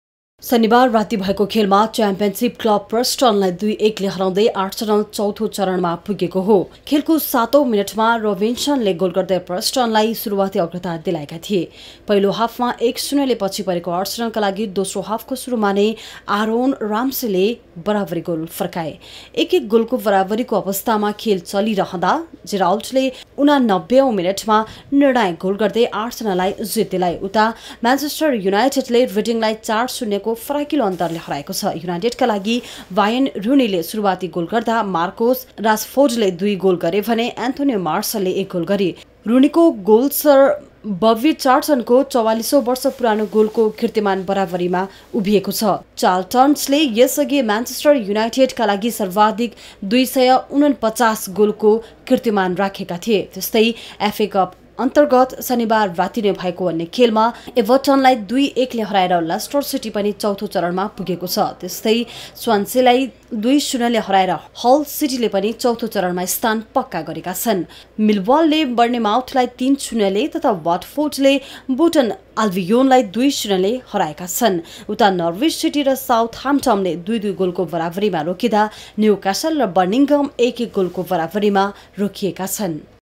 वार्यन रुनीले गरे म्यान्चेष्टर युनाईटेडका लागि बबी चाल्र्टनको बराबरी गोल -NEWS 24
एफए कप फुटबल प्रतियोगिता अन्र्तगत आर्सनल अघिल्लो चरणमा प्रवेश गरेको छ । निकै लयमा फर्किएको म्यानचेष्टर युनाईटेडले भने रिडिङलाई चार शून्यको फराकिलो अन्तरले हराउदै युनाईटेड  पनि एफए कपको चौथो चरणमा प्रवेश गरेको छ ।शनिबार राति भएको खेलमा च्याम्पियनशीप क्लब प्रिस्टनलाई २–१ ले हराउँदै आर्सनल चौथो चरण पुगेको हो । खेलको सातौं मिनेटमा रोबिन्सनले गोल गर्दै प्रिस्टनलाई सुरुवाती अग्रता दिलाएका थिए । पहिलो हाफमा १–० ले पछि परेको आर्सनलका लागि दोस्रो हाफको सुरुमा नै आरोन रामसीले बराबरी गोल फर्काए । एक एक गोलको बराबरी अवस्थामा खेल चलिरहदा जिरउडले ८९ औं मिनेटमा निर्णायक गोल गर्दै आर्सनललाई जित दिलाए । उता म्यानचेस्टर युनाइटेडले रिडिङलाई ४–० को फराकिलो अन्तरले हराएको छ । युनाईटेडका लागि वार्यन रुनीले सुरुवाती गोल गर्दा, मार्कोस रासफोर्डले दुइृ गोल गरे भने एन्थोनी मार्सलले एक गोल गरे । रुनीको गोल सर वबी चाल्र्टनको ४४ बर्ष पुरानो गोलको कीर्तिमान बराबरीमा उभिएको छ । चाल्टर्नले यस अघि म्यान्चेष्टर युनाईटेडका लागि सर्वाधिक २४९ गोलको कीर्तिमान राखेका थिए । यस्तै एफ कप अन्र्तगत शनिबार राति नै भएको अन्य खेलमा एभर्टनलाई २–१ ले हराएर लेस्टर सिटी पनि चौथो चरणमा पुगेको छ ।